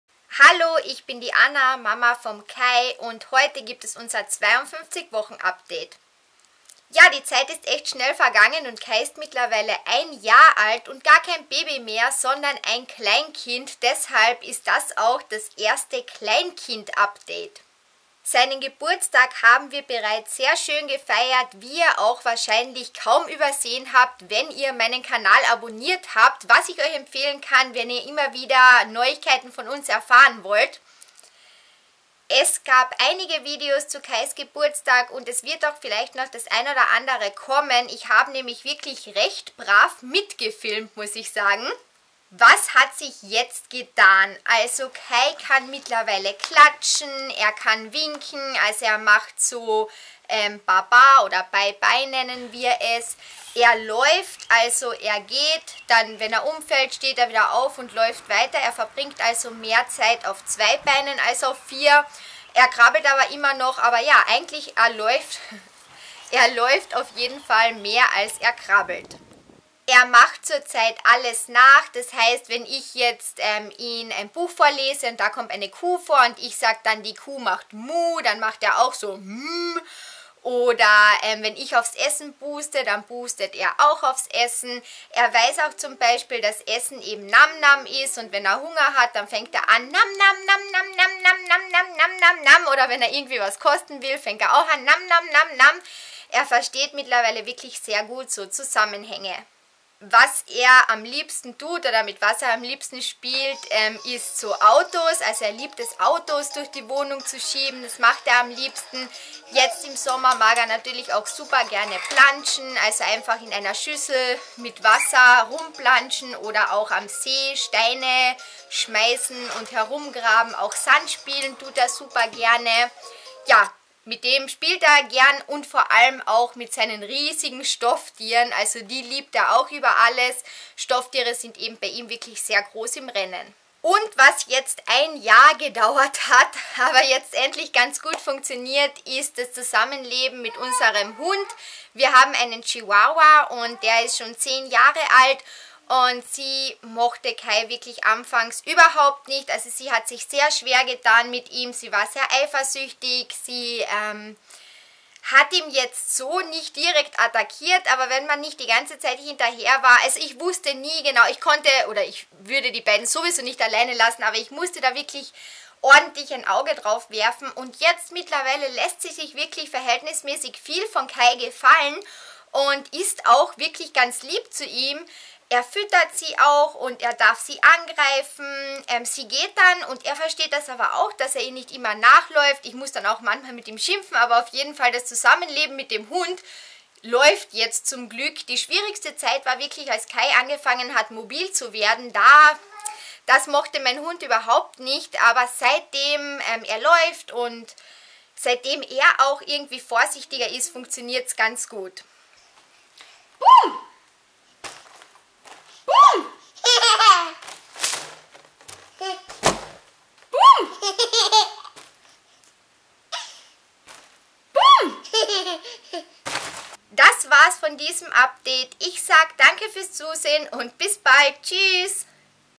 Update Mama & Kleinkind: Kai ist 1 Jahr alt | Babyartikel.de
Wow, jetzt ist Kai schon 1 Jahr alt. Was Kai alles schon kann und womit er gerne spielt, erzähle ich Euch im Update.